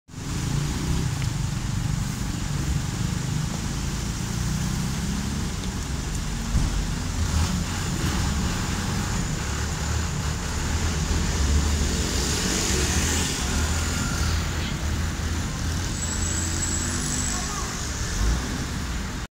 Επιχείρηση εκκαθάρισης Αγίου Δημητρίου - GRTimes.gr
Συντονισμένη επιχείρηση "εκκαθάρισης" της Αγίου Δημητρίου από τα παράνομα παρκαρισμένα ΙΧ πραγματοποίησε σήμερα το πρωί η Τροχαία Θεσσαλονίκης και ο κεντρικός Δήμος.